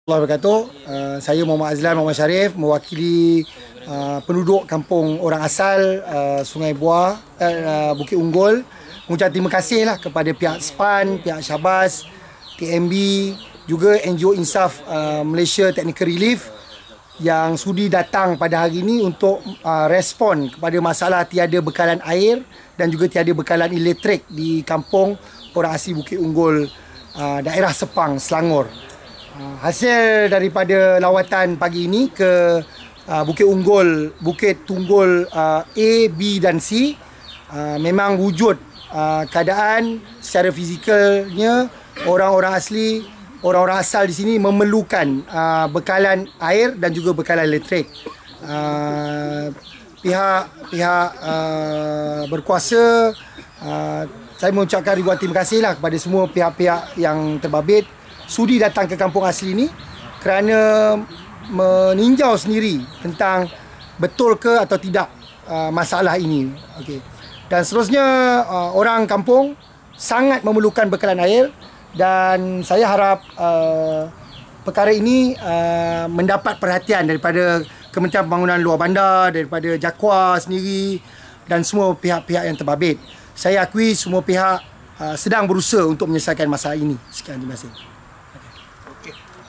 TNB, SYABAS, SPAN turun padang selepas pendedahan FMT
Syarikat utiliti seperti Tenaga Nasional Berhad (TNB), Syarikat Bekalan Air Selangor (SYABAS) dan Suruhanjaya Perkhidmatan Air Negara (SPAN) membuat kunjungan ke kampung-kampung orang asal kawasan Dengkil selepas laporan media yang dibuat oleh FreeMalaysiaToday.